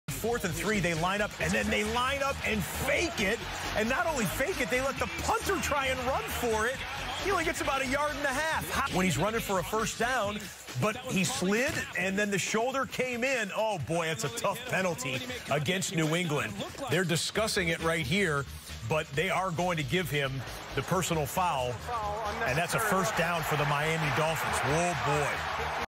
One of the worst calls you'll see
The giants appear to be tanking as the run a QB sneak on 3rd and 9 from their own 4 yard line. Saddest NFL play in a while.